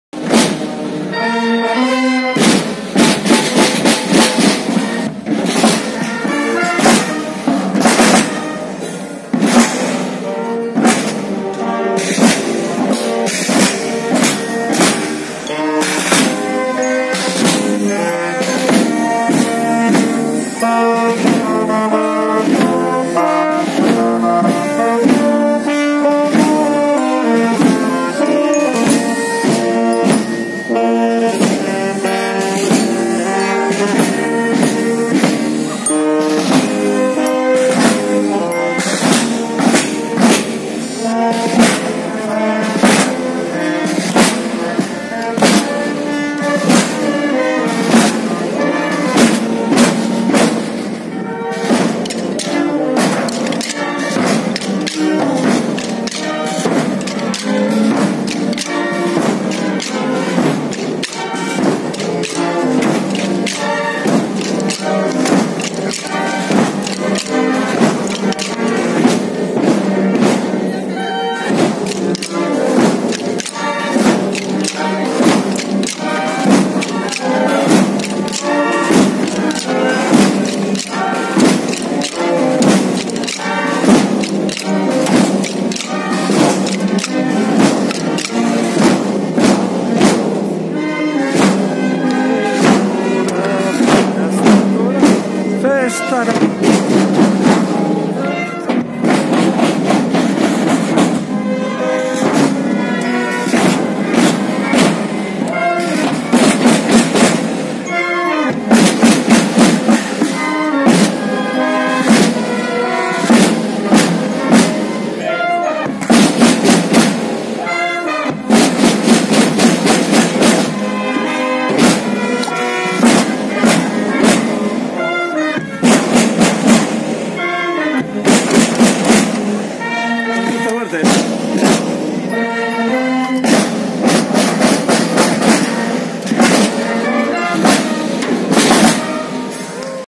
Macachin 2022 EAN-SNV Danborrada 02
Danborrada Macachin 2022 Euskal Aste Nazionalean
Tamborrada en la Semana Nacional Vasca de Argentina